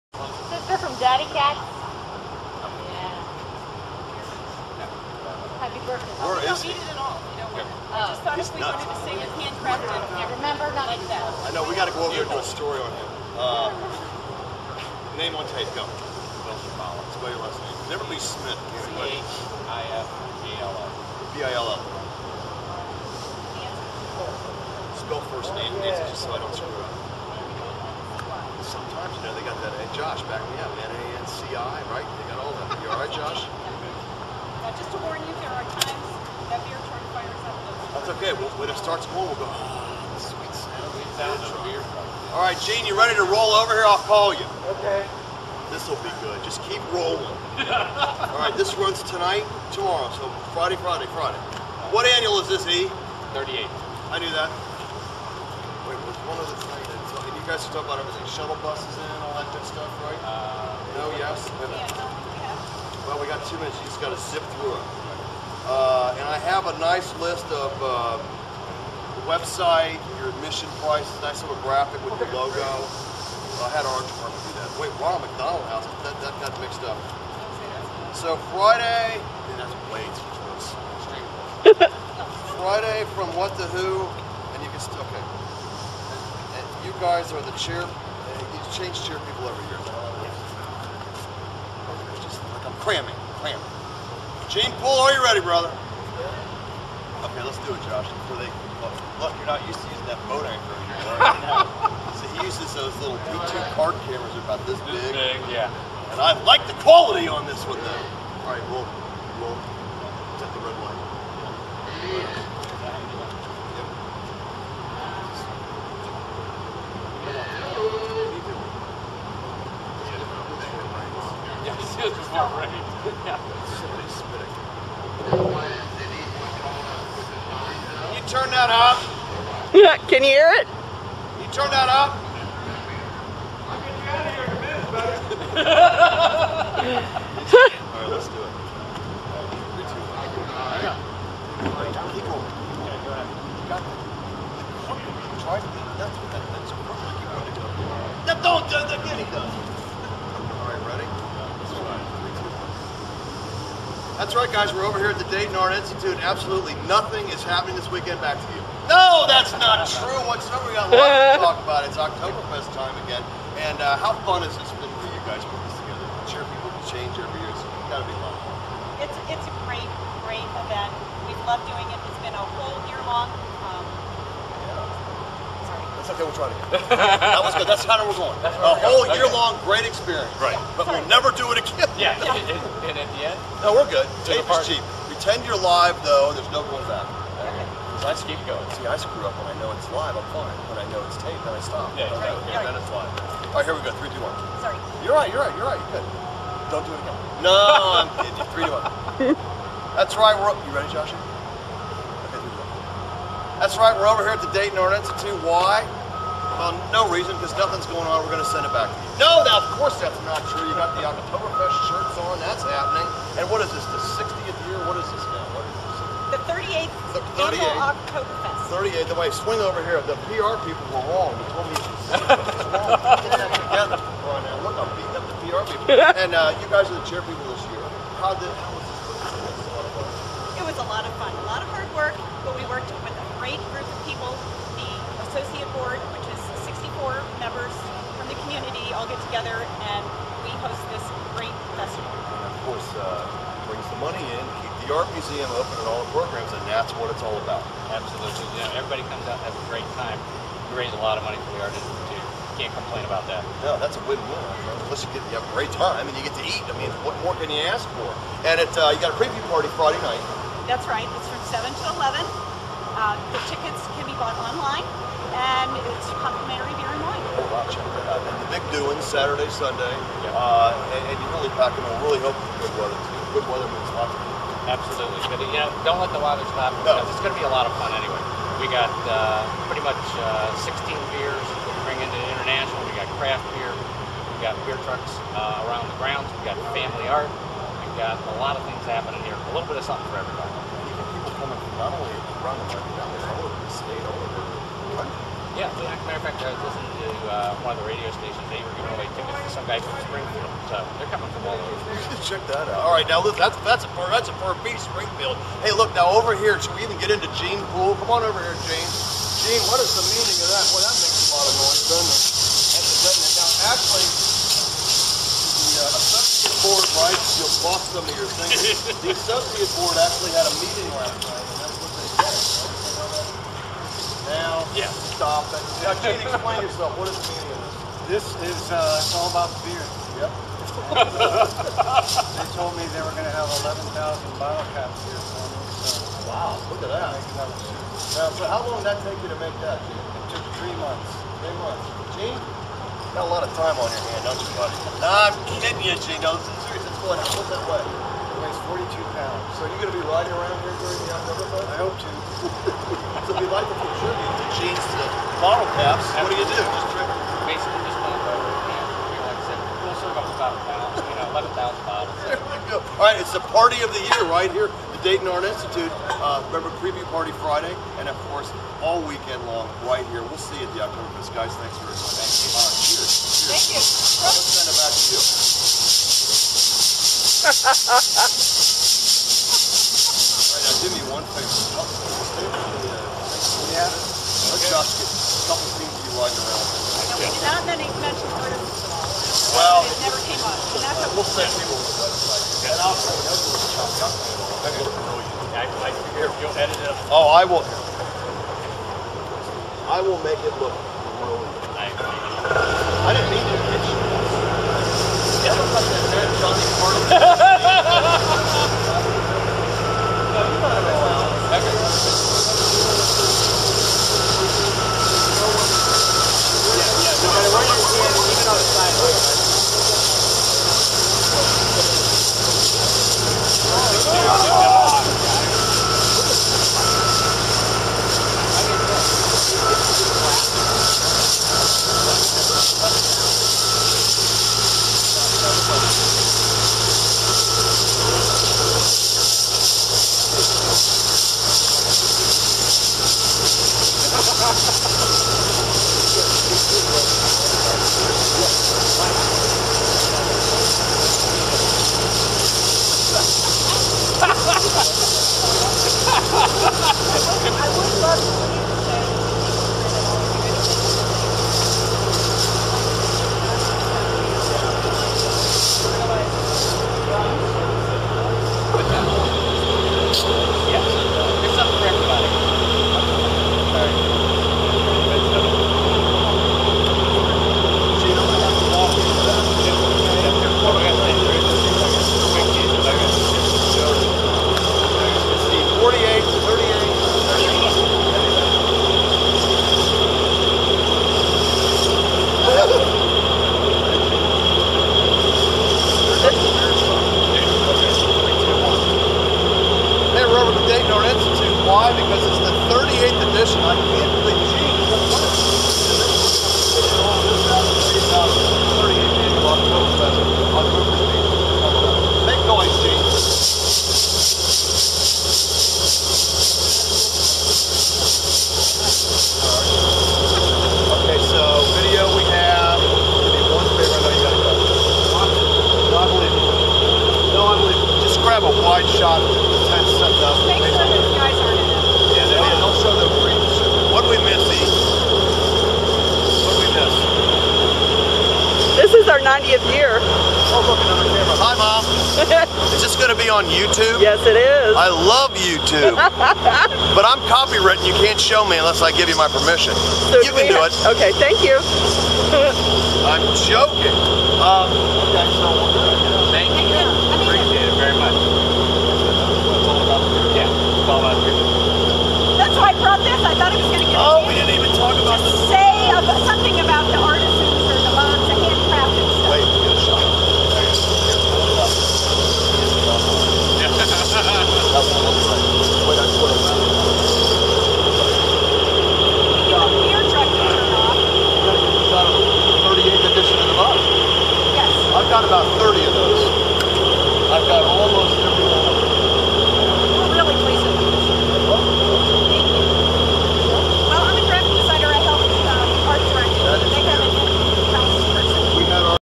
Dayton Art Institute Oktoberfest: Behind the Scenes with Jim Bucher
Behind the scenes with WDTN's Jim Bucher at The Dayton Art Institute, as we record a "Bucher's Beat" segment about Oktoberfest 2009. Featured are Oktoberfest Chairs Bill & Nancy Chifala, along with Gene Pool and his suit made from 10,000 beer bottle caps!